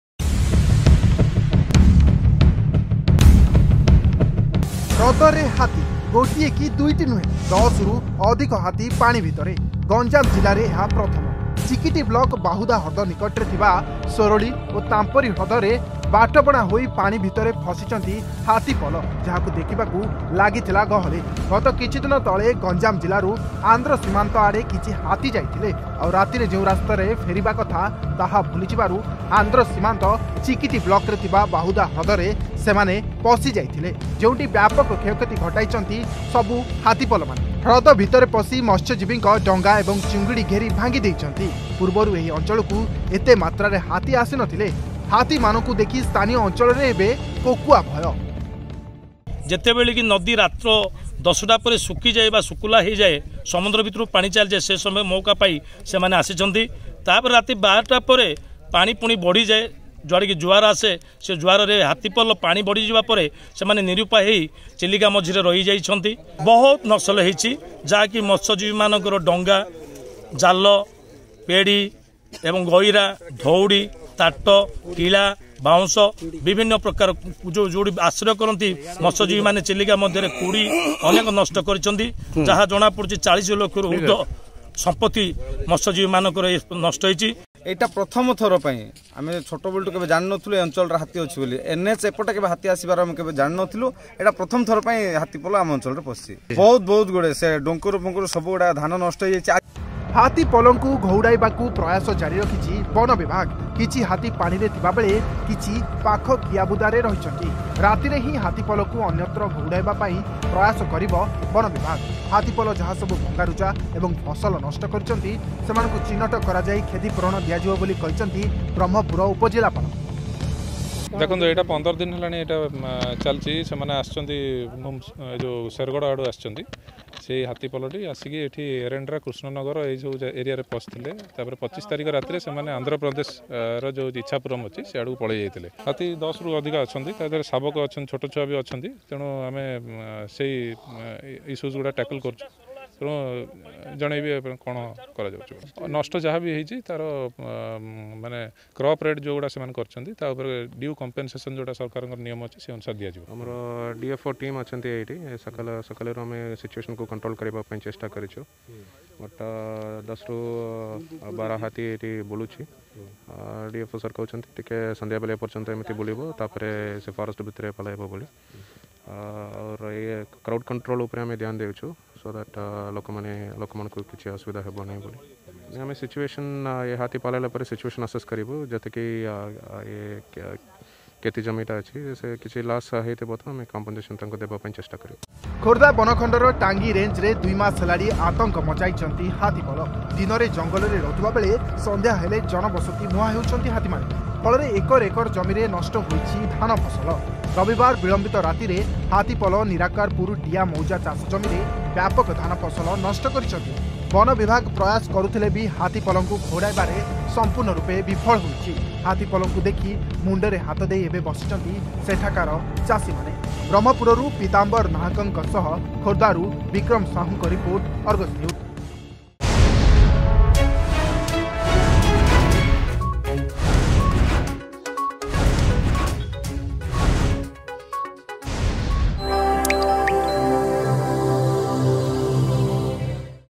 ହ୍ରଦରେ ହାତୀ । ଗୋଟିଏ କି ଦୁଇଟି ନୁହେଁ, 10ରୁ ଅଧିକ ହାତୀ ପାଣି ଭିତରେ
ହ୍ରଦରେ ହାତୀ । ଗୋଟିଏ କି ଦୁଇଟି ନୁହେଁ, 10ରୁ ଅଧିକ ହାତୀ ପାଣି ଭିତରେ । ଗଞ୍ଜାମ ଜିଲ୍ଲାରେ ପ୍ରଥମ । ଚିକିଟି ବ୍ଲକ ବାହୁଦା ହ୍ରଦ ନିକଟରେ ଥିବା ସୋରଳି ଓ ତାମ୍ପରି ହ୍ରଦରେ ବାଟବଣା ହୋଇ ପାଣି ଭିତରେ ପଶିଛନ୍ତି ହାତୀ ପଲ ..